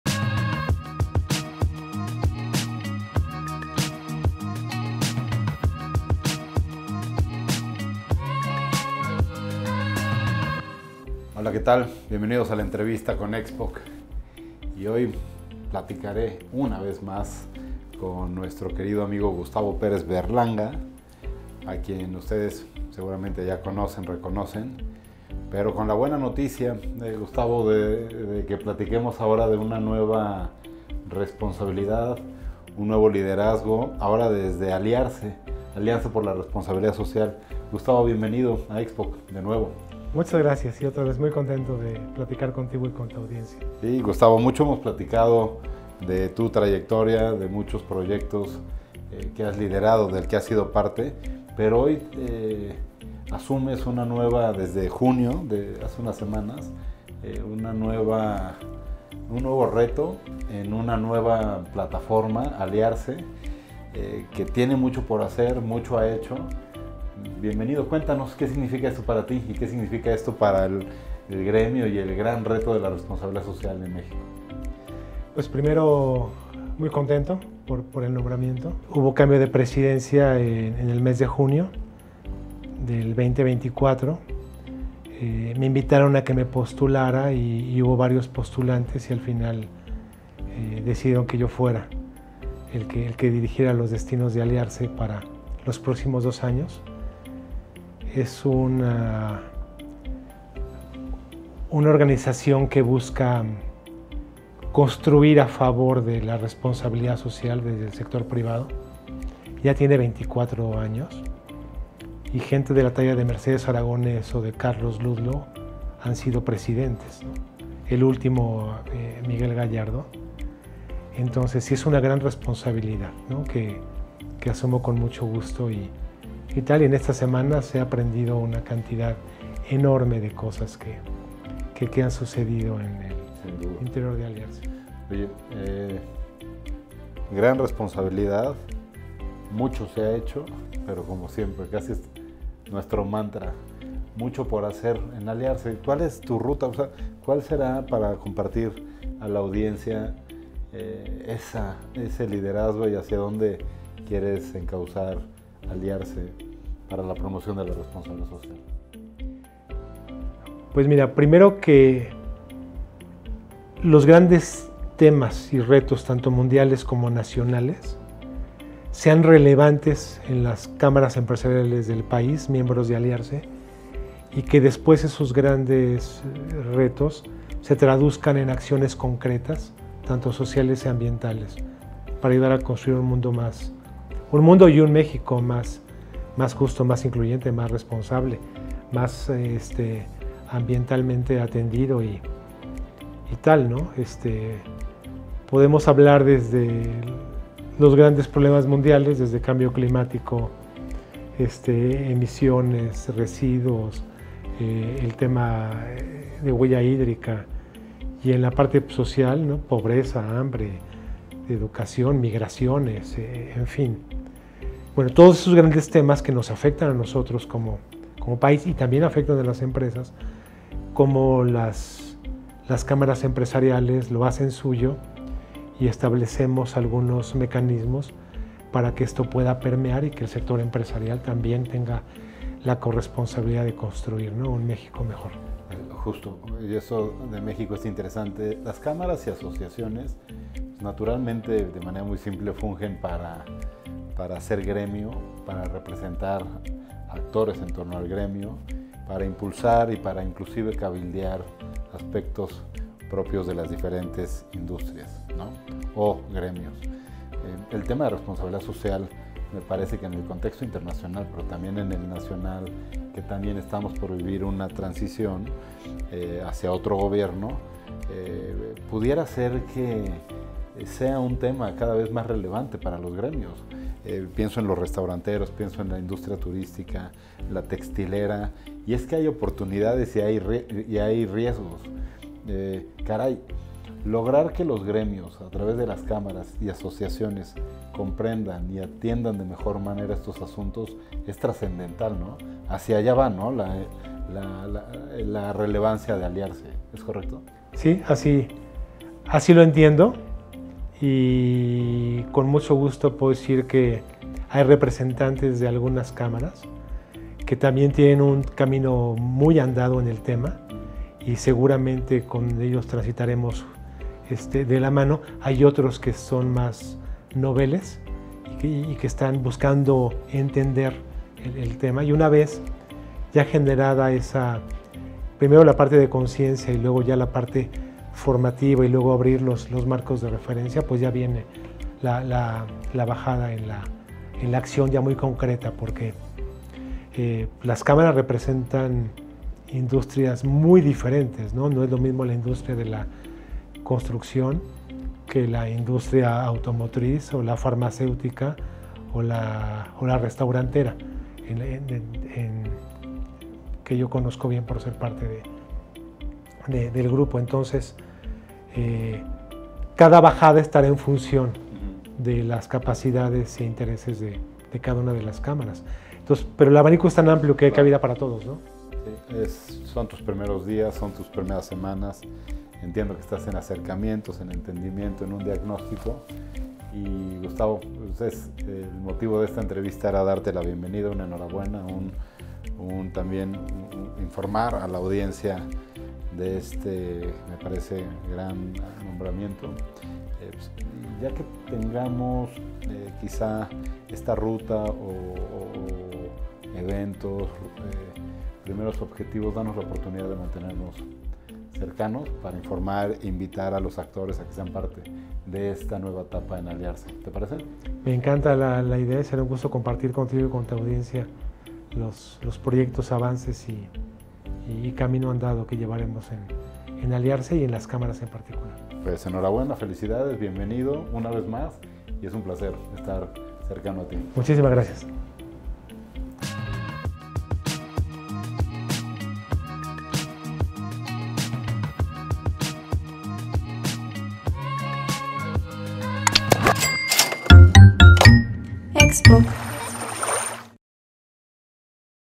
La Entrevista con Gustavo Pérez, nuevo Presidente de AliaRSE
De nueva cuenta, Edgar López entrevista a Gustavo Pérez, el nuevo presidente de AliaRSE, quien nos comparte detalles sobre su reciente nombramiento y su visión para la organización. Gustavo, con una destacada trayectoria en el ámbito de la responsabilidad social empresarial, nos ofrece una perspectiva fresca y motivadora sobre su rol y los retos que enfrenta.

Durante la charla, Gustavo nos habla sobre los principales objetivos que se ha propuesto a corto plazo, destacando la importancia de fortalecer las alianzas con empresas y organizaciones para impulsar iniciativas sostenibles. Su enfoque en la gestión actual se centra en la promoción de prácticas responsables y el desarrollo de proyectos que generen un impacto positivo en la comunidad.

No te pierdas esta enriquecedora conversación donde Gustavo Pérez comparte su pasión por la responsabilidad social y su compromiso con AliaRSE. Conoce más sobre sus estrategias y planes para llevar a la organización a nuevos horizontes, promoviendo un cambio significativo y duradero en la sociedad.